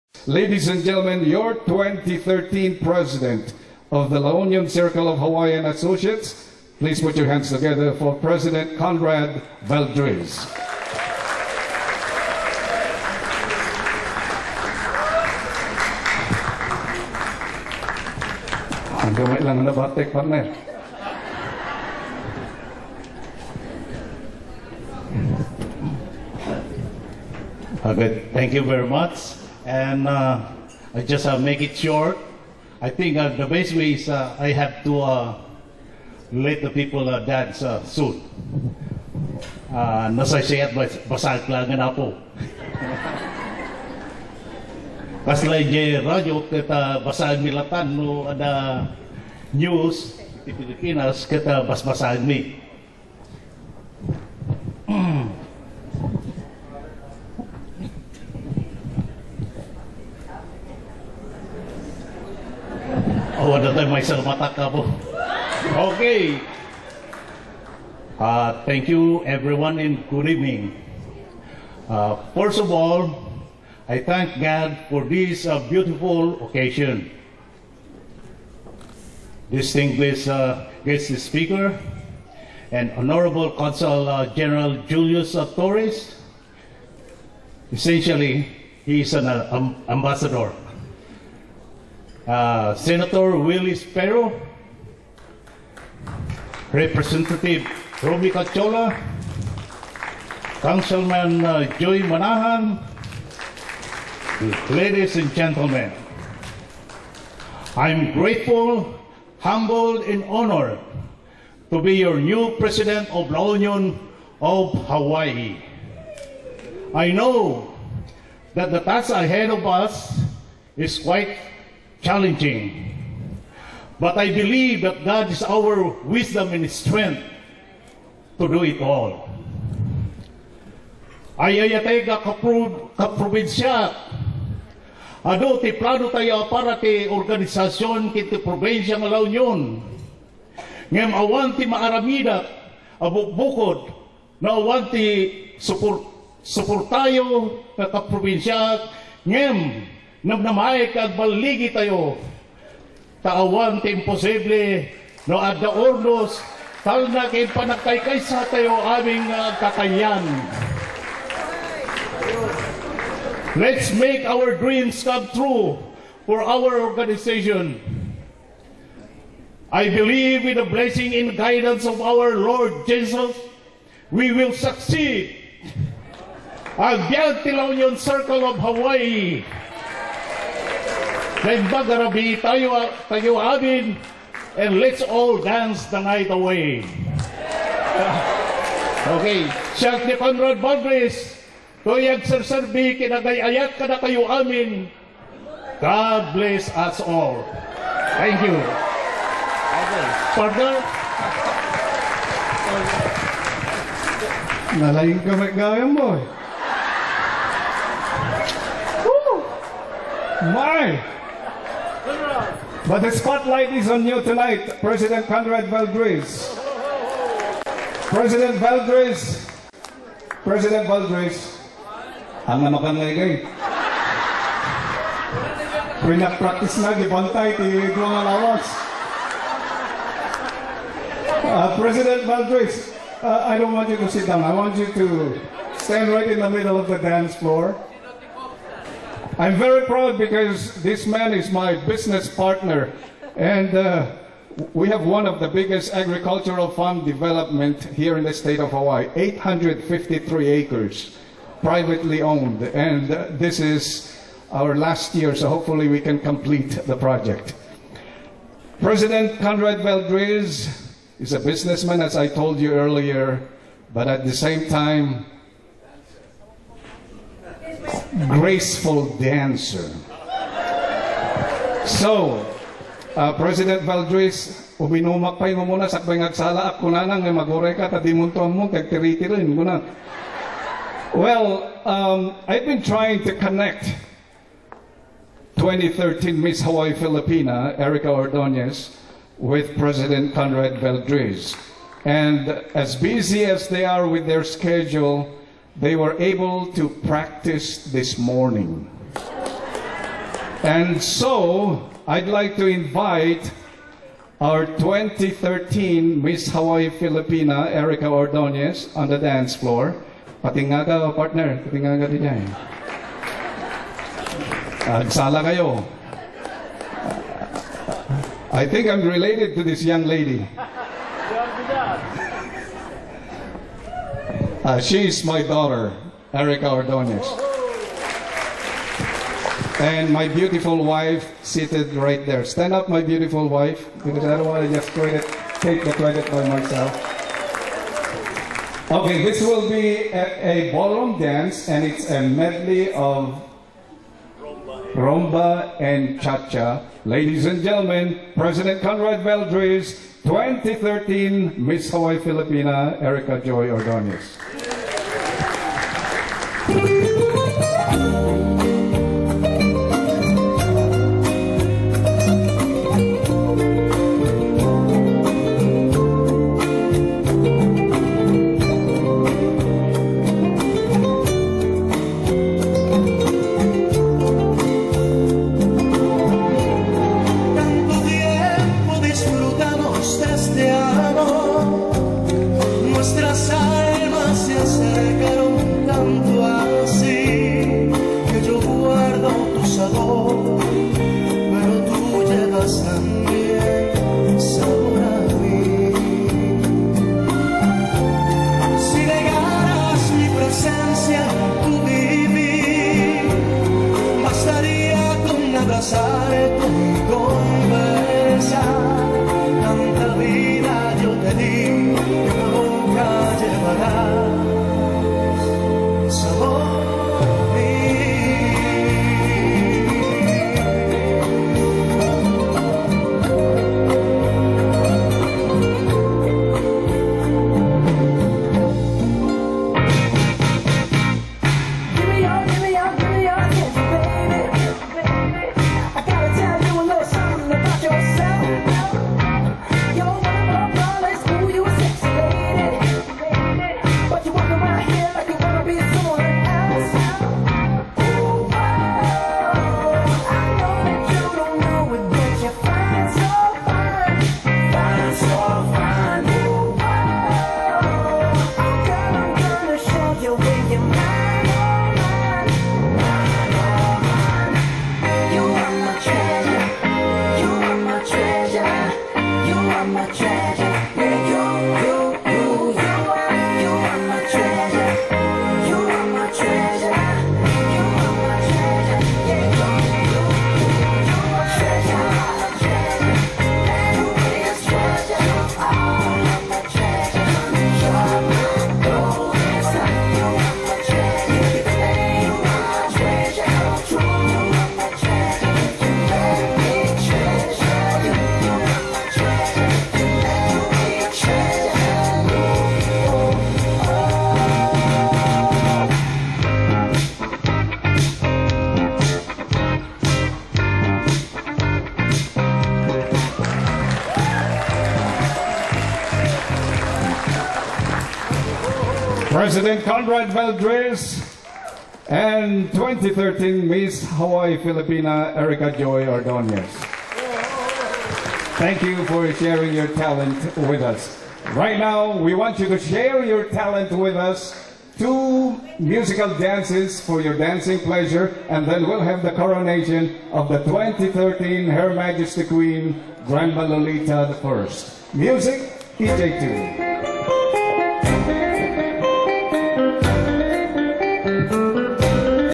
Conrad Valdriz Speech La Union Circle of Hawaii 2013
Acceptance Speech by Conrad Valdriz, 2013 President of the La Union Circle of Hawaii at the Installation of Officers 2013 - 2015 held Ala Moana Hotel in Honolulu, Hawaii on September 21, 2013